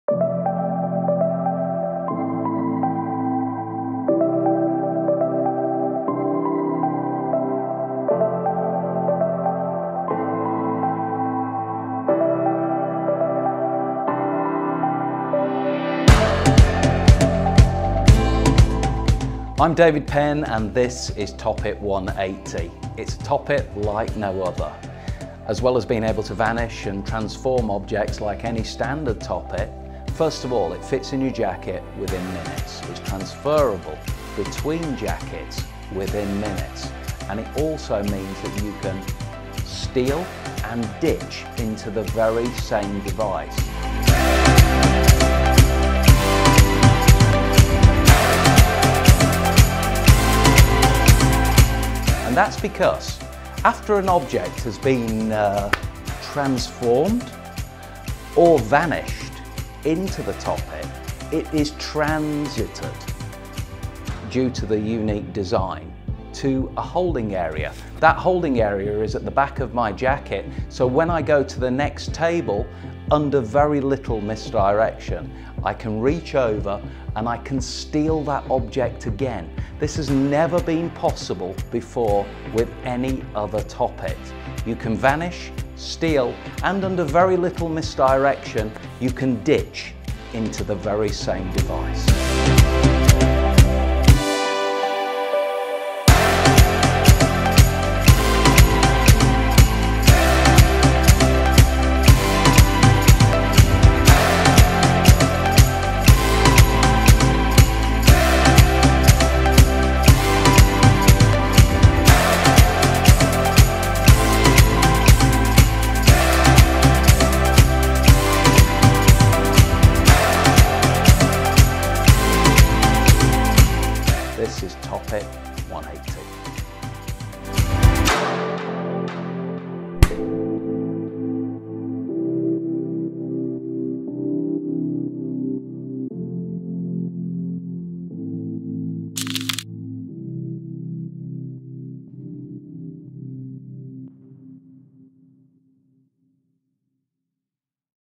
Topit 180 by David Penn - Official Trailer
World Magic Shop DO NOT SELL Every product listed anymore. for the items we sell. Quality not Quantity :-) "If real magic existed I am sure this is what it would look like!

David Penn has taken this utility device to the next level.

If you want to improve your magic get this gimmick!

You won't TOP IT!"

Wayne Dobson

"Revolutionary!"
Doc Eason

“David Penn's Topit 180 has multiple uses removing all the limitations of standard Topits. It will help performers to create the best visual magic possible.”

Brendan Rodrigues

“David Penn has created effects with the Topit that no one else ever dreamed possible”
Patrick Page

For the first time ever with David Penn’s revolutionary design of Topit……..Vanish, Steal & Ditch.

David Penn has lectured all over the world on his natural technique with a Topit. Recognised as a modern master, of this sometimes forgotten utility device, David now presents his evolution of this incredible gimmick - TOPIT 180.

With TOPIT 180 you can vanish a small object at any time! This can be presented as an effect or to covertly ‘finish clean’ after an effect that requires a gimmick.

As described, and shown on the trailer, the very same object can then be retrieved under very little misdirection after it is transited to a holding area, which is all built into TOPIT 180. This very same area can be used to ditch objects covertly giving you so much freedom in your routining.

If you are familiar with a topit you will revel in these new possibilities however if you are new to topitting David covers obsoletely everything in the comprehensive download that you are given access to.

As well as the techniques involving vanishing, stealing and ditching small objects, you will be taught effects with: Metal Bending,Candles , 8 Ball Transformations, Glass Restorations, Coin In Bottle and Salt Pot Vanishes.

TOPIT 180 is the ultimate TOPIT!